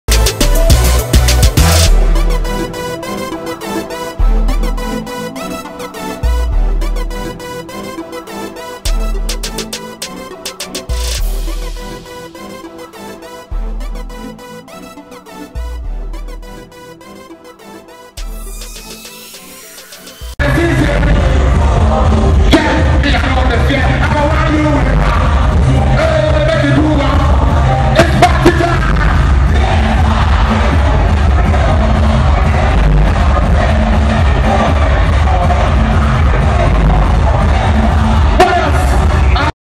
Sarkodie - Dancing 'Alkayida' @ Vodafone Icons: Street Edition | GhanaMusic.com Video
PLEASE NOTE: Unauthorized upload of this video unto any YouTube channel is prohibited. (C) 2013. MiPROMO Media

Check out Sarkodie's dance moves of 'Alkayida'.

Ghana Music•com™ • Just log